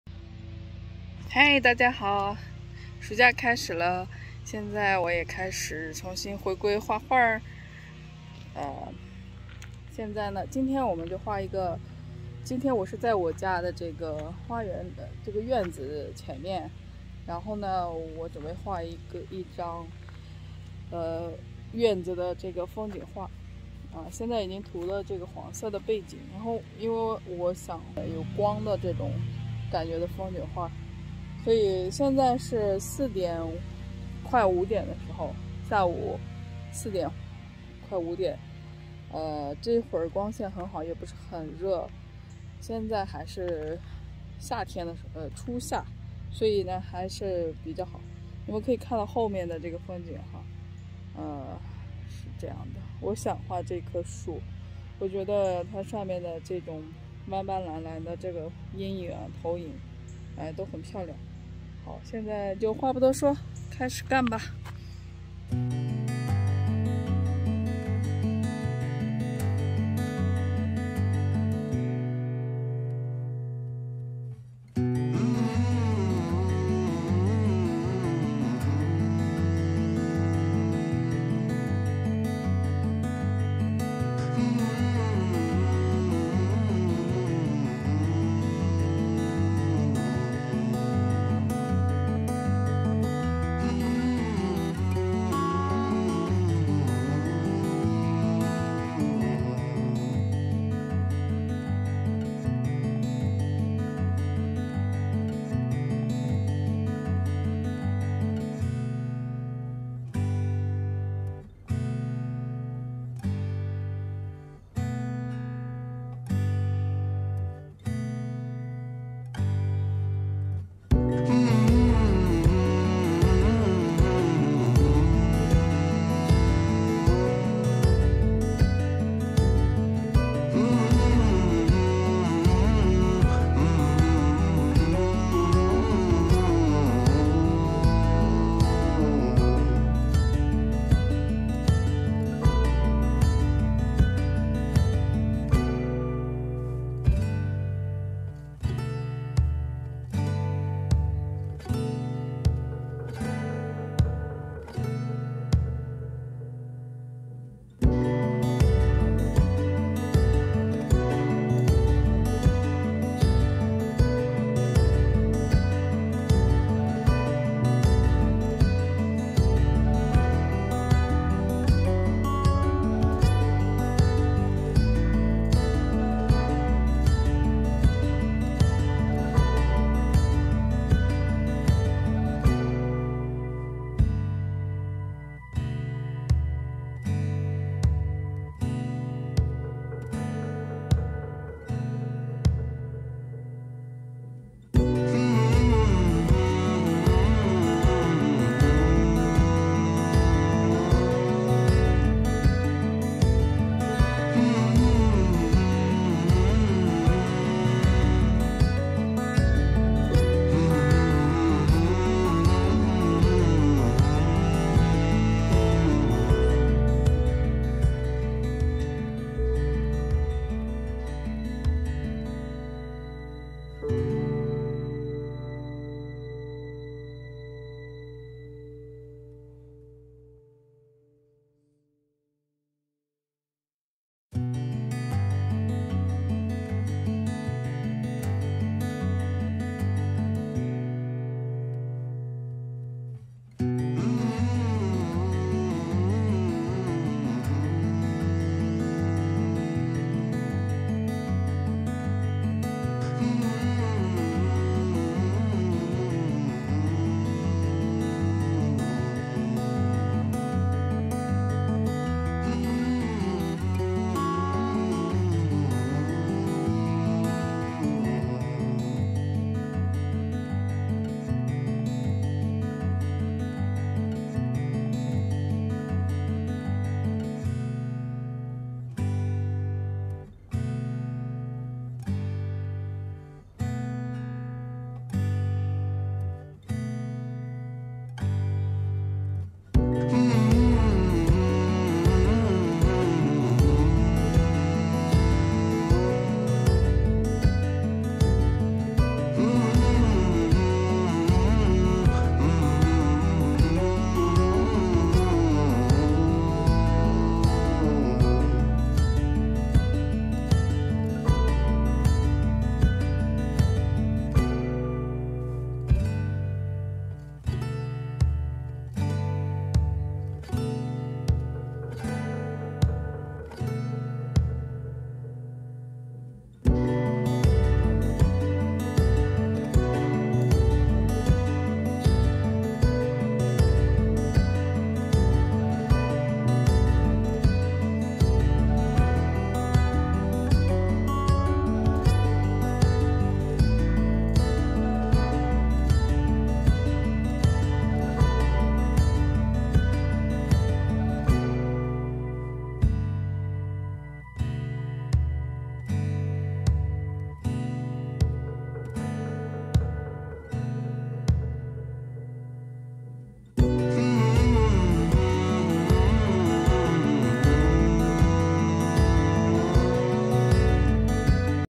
初夏写生20220529 风景写生 绘画过程 watercolor 艺世界 painting process
2022年初夏第一张户外写生，就在自己家院子里，耗时三个小时，中间碰上邻居画家老太太聊天一小时，哈哈哈，户外写生天气、器材、光线、时间、和好奇的人都是不可抗力因素。 @艺世界  @ELKAUDIOBOOK